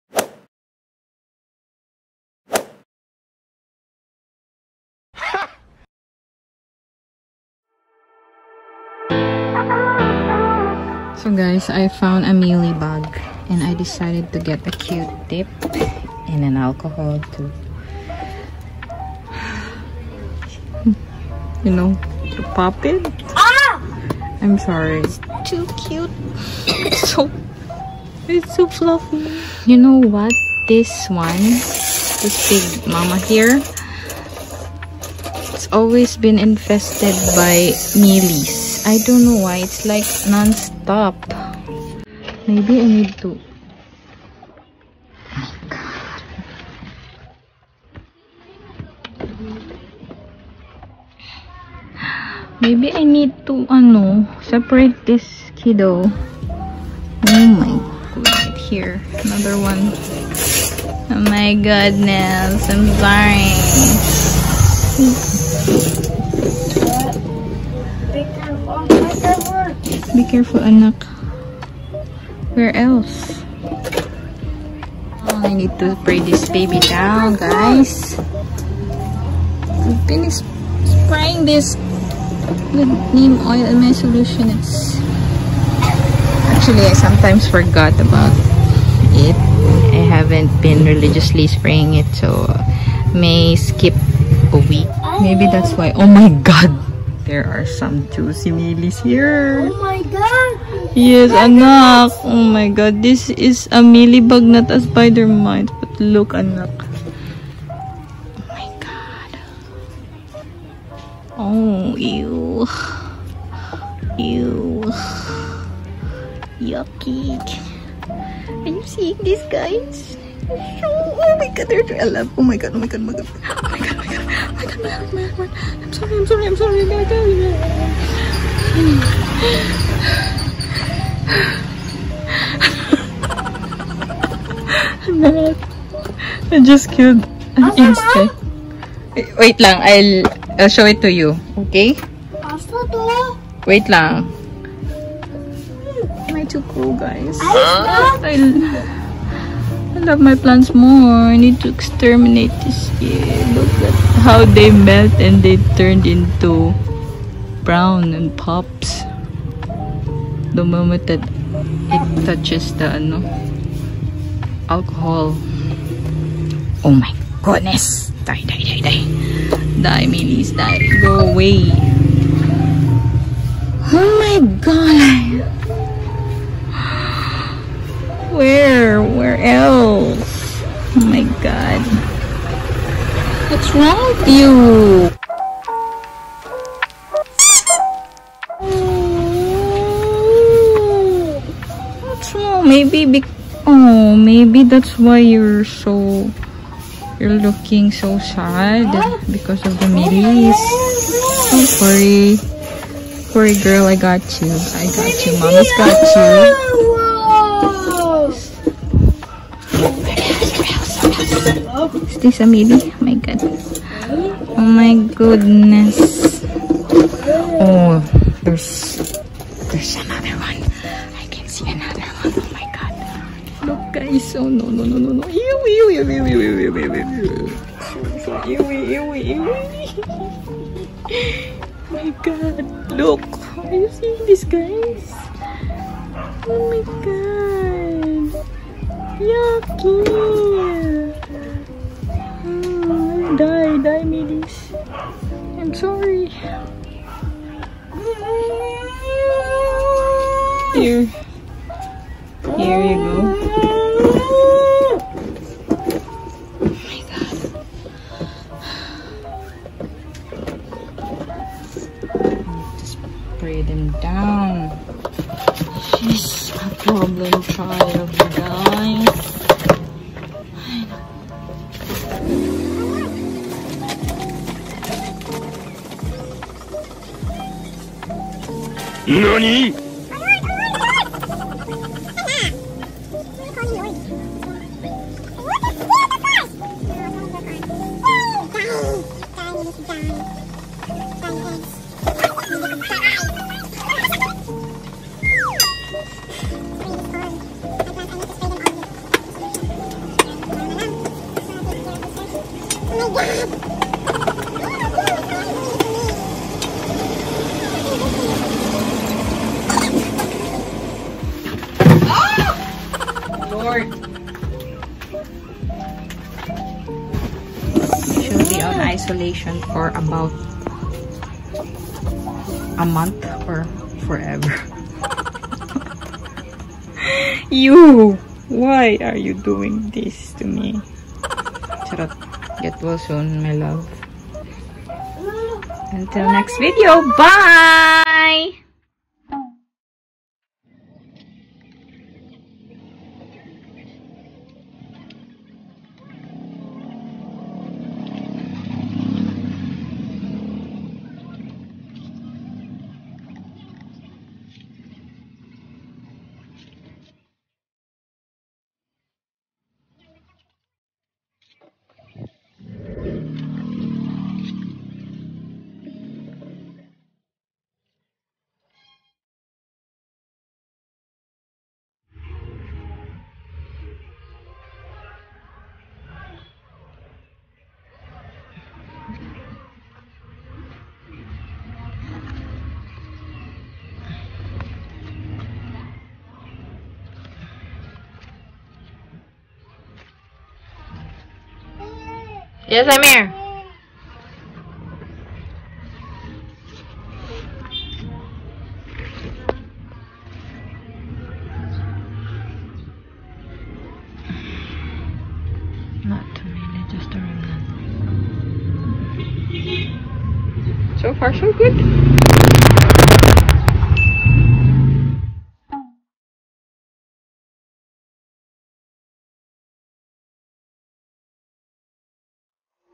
ACTUAL MEALYBUG REMOVAL| Pest control on your plants | Syngonium Pink Vein + Results
It’s always a pain whenever you discover unwelcome guests to your household. You should know how much my insides hurt when a mealybug caught my eyes uggh! So gross 🤢 .

My syngonium pink vein has always been a problem child for me. In this video you’ll see how infested my baby is and how I remove all the mealybugs and stop it from spreading . Pest management has been too hard for me since the beginning of my plant journey and I think you can never really get rid of them forever . It is just a matter of controlling it by prevention.

Music